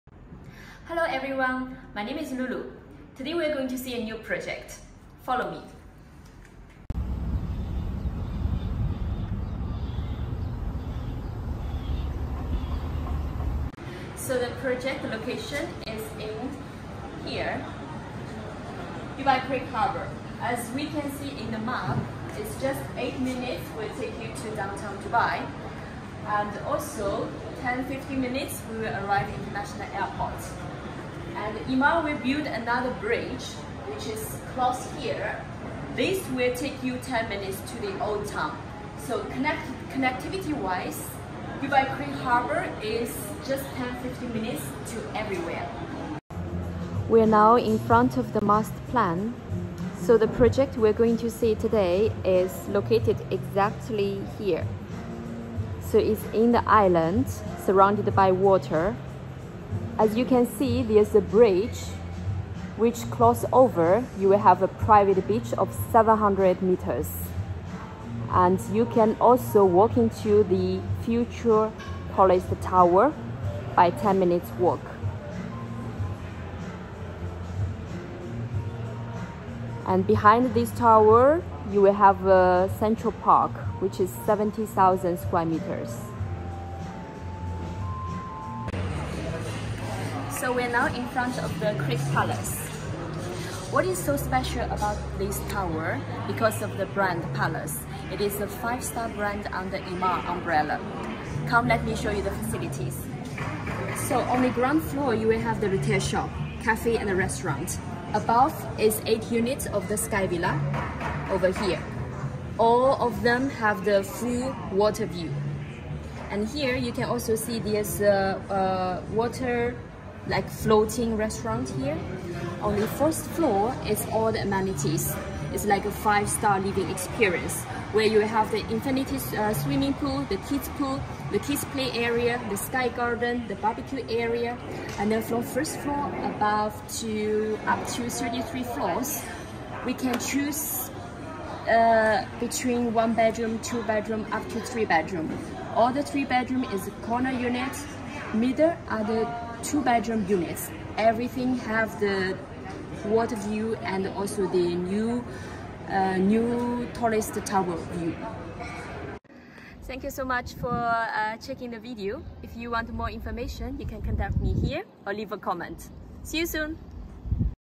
Creek Palace - Dubai Creek Harbour
Creek Palace is a five star water side residence, located in the prime spot of Dubai Creek Island. It is few mins walking distance to a lot of world popular amenities. Behind the residence it is a huge Central Park sized 70,000 sqm with greenery and fun, in front of the residence you have cafe, restaurant and water floating F&B avenue. Cross the pedestrian bridge you are on a private man made beach 700 meters long, further down you can walk towards the new tallest tower in the world and the worlds biggest shopping center. And steps always you have creek marina and five star hotels like Palace hotel, Address Hotel..Creek Palace comes with a 3 basement parking, plus a ground floor huge grand lobby, waterfront shopping and dinning experience downstairs, slightly above you have 8 exclusive villas facing the beautiful canal. On first floor it comes with all the amazing facilities like a resort style living. Big infinity pool, kids pool and play area, sky garden, BBQ, gym.. you name it… if it’s not enough you go next door is luxurious five star hotel Place, where you can dine and have a sip of good wine, nice spa.. with discounted price. Also valet parking, concierge, housekeeping, in room dining is just a call away, with additional pay for it. Life here is just absolutely beautiful. You can choose to invest from 1bedroom up to 3 bedroom in this project. Payment plan is super flexible as well. Just a few years time you shall see how the value appreciates.